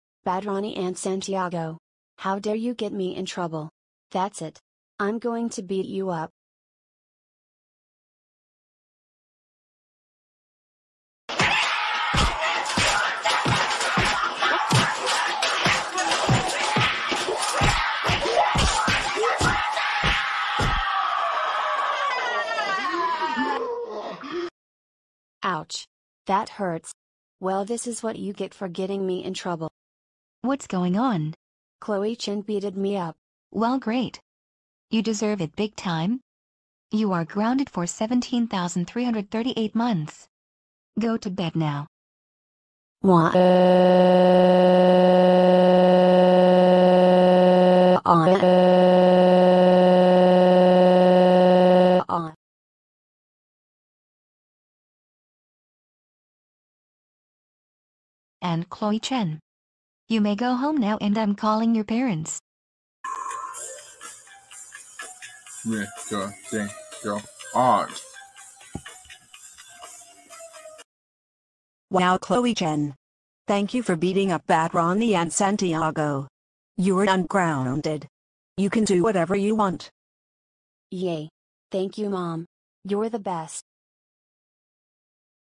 Chloe Chen Beats Up Bad Ronnie Anne Santiago And Gets Ungrounded
The Chens Gets Ungrounded